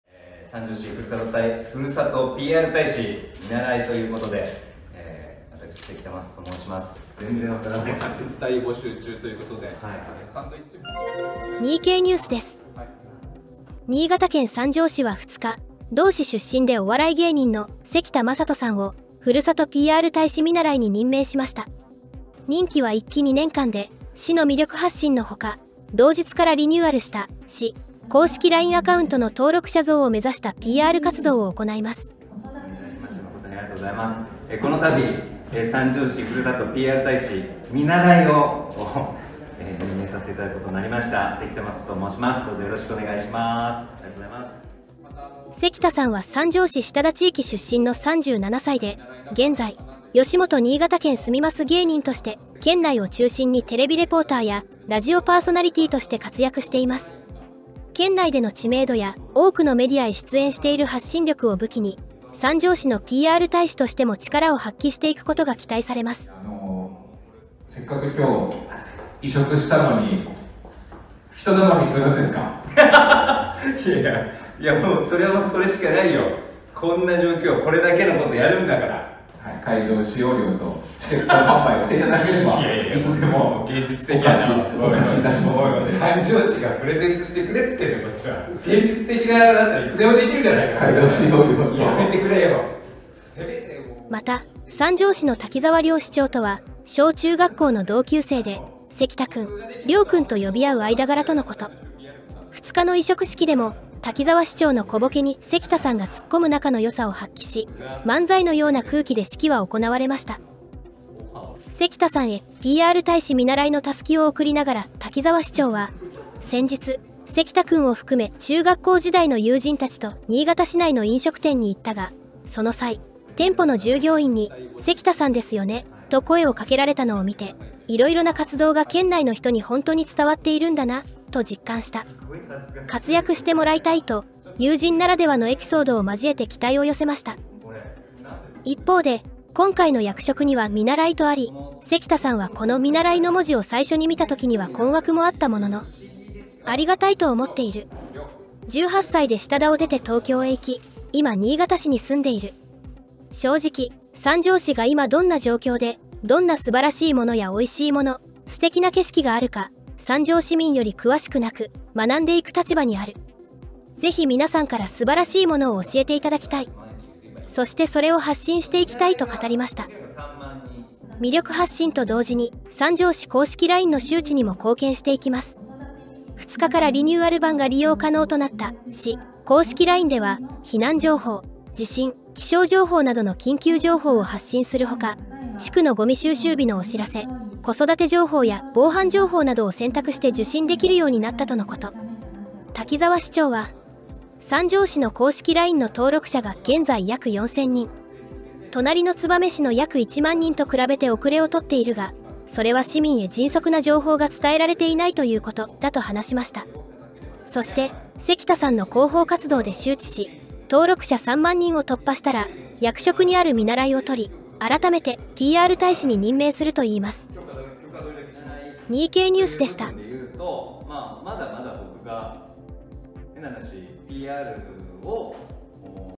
【よしもと新潟県住みます芸人】新潟県三条市が下田地域出身のお笑い芸人・関田将人さんをふるさとPR大使「見習い」に任命【目指せ３万人、市公式LINEアカウント登録者増】
新潟県三条市は２日、同市出身でお笑い芸人の関田将人さんをふるさとPR大使見習いに任命した。

関田さんは三条市下田地域出身の３７歳。現在「よしもと新潟県住みます芸人」として県内を中心にテレビレポーターやラジオパーソナリティとして活躍している。県内での知名度や多くのメディアへ出演している発信力を武器に、三条市のPR大使としても力を発揮していくことが期待される。

また、三条市の滝沢亮市長とは小・中学校の同級生で、「関田くん」「亮くん」と呼び合う間柄でもある。２日の委嘱式でも、滝沢市長の小ボケに関田さんがツッコむ仲の良さを発揮し、漫才のような空気で式は行われた。

関田さんへPR大使見習いのたすきを贈りながら滝沢市長は「先日、関田くんを含め中学校時代の友人たちと新潟市内の飲食店に行ったが、その際、店舗の従業員に『関田さんですよね』と声をかけられたのを見て、『（関田さんの）色々な活動が県内の人に本当に伝わっているんだな』と実感した。（PR活動で）活躍してもらいたい」と友人ならではのエピソードを交えて期待を寄せた。

一方で、今回の役職には「見習い」とある。関田さんはこの「見習い」の文字を最初に見た時には困惑もあったものの「（むしろ）ありがたいと思っている。１８歳で下田を出て東京へ行き、今新潟市に住んでいる。正直、三条市が今どんな状況で、どんな素晴らしいものや美味しいもの、素敵な景色があるか、三条市民より詳しくなく、学んでいく立場にある。ぜひ皆さんから素晴らしいものを教えていただきたい。そしてそれを発信していきたい」という。

２日からリニューアル版が利用可能となった市公式LINEでは、避難情報、地震、気象情報などの緊急情報を発信するほか、地区のごみ収集日のお知らせ、子育て情報や防犯情報などを選択して受信できるようになった。

隣の燕市の約１万人と比べて遅れをとっているが、それは市民へ迅速な情報が伝えられていないということ」だと話す。そして、関田さんの広報活動で周知し、登録者３万人を突破したら役職にある「見習い」を取り、改めて「PR大使」に任命するという。

【にいがた経済新聞】にいがたをもっと身近に。